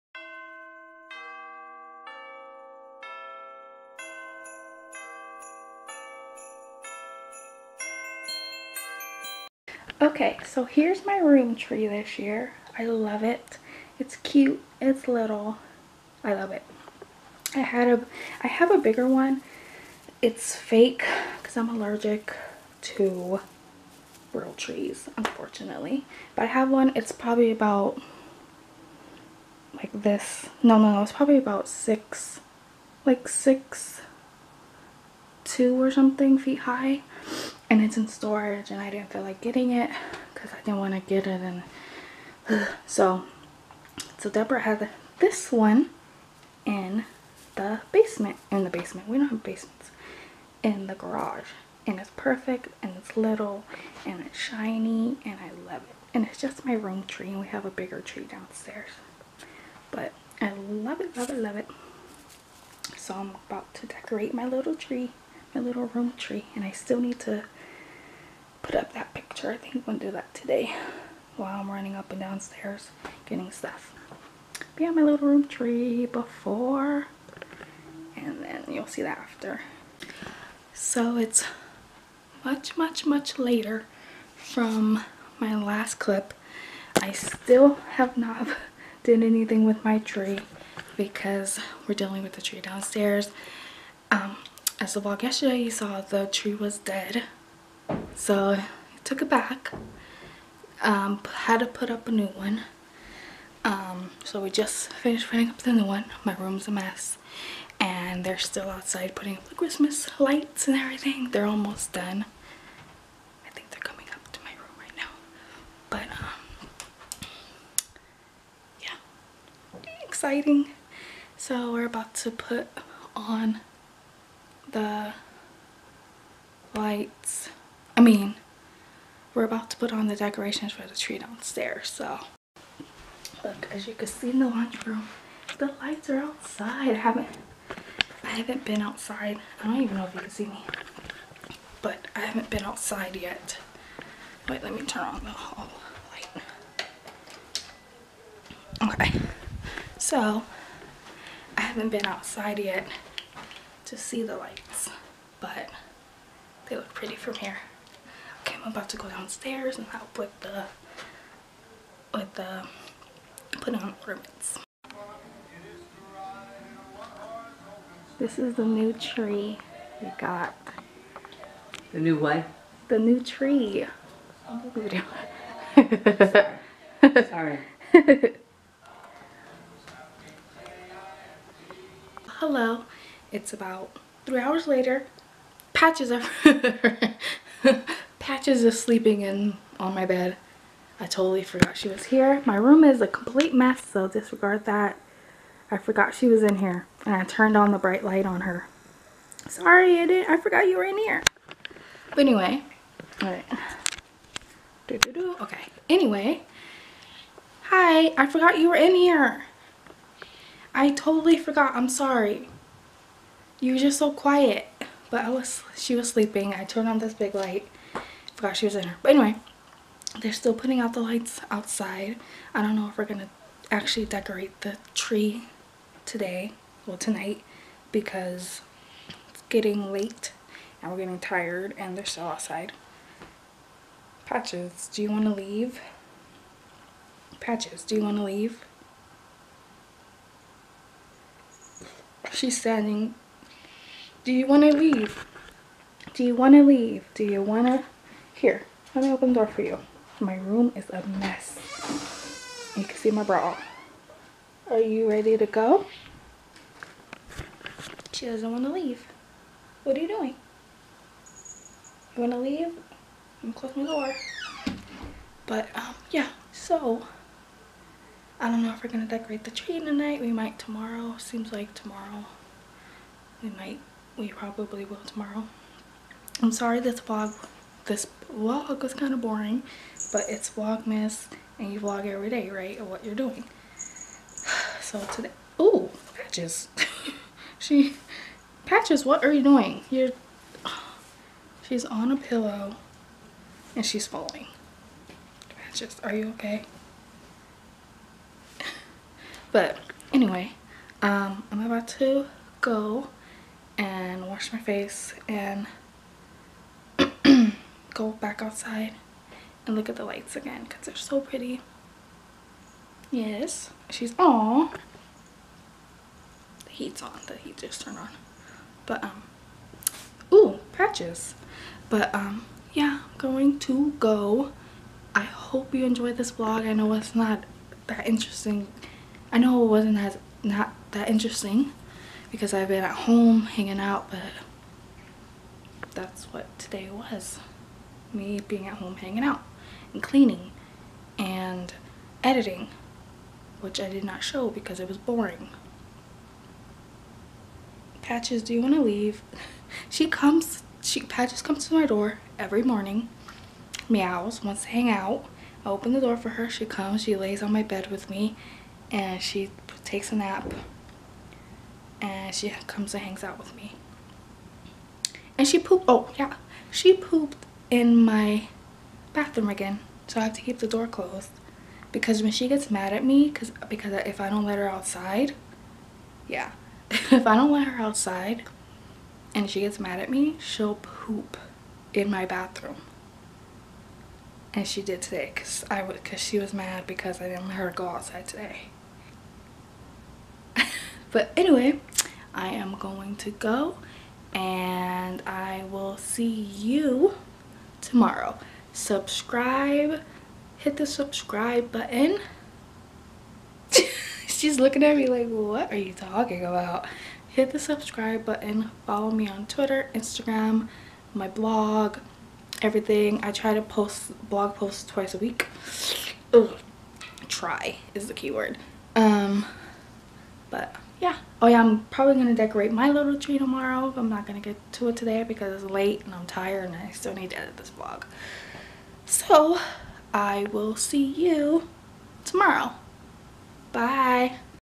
WE GOT THE TREE | Vlogmas Day 2
After our first tree died. We got a 2nd tree.